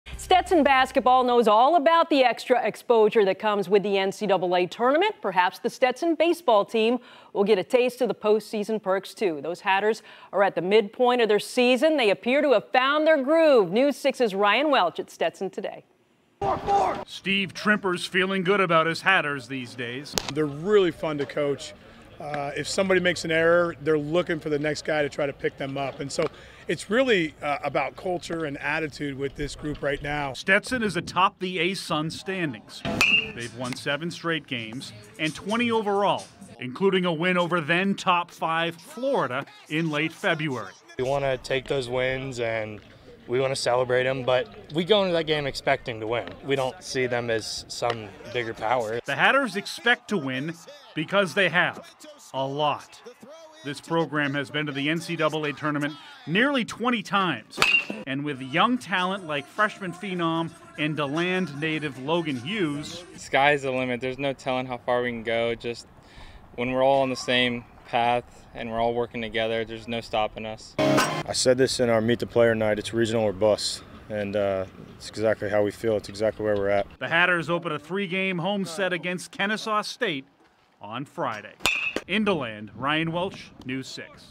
Stetson baseball team tops ASUN Conference heading into home set against Kennesaw State
Stetson is atop the ASUN standings as the Hatters open a three-game home set against Kennesaw State on Friday.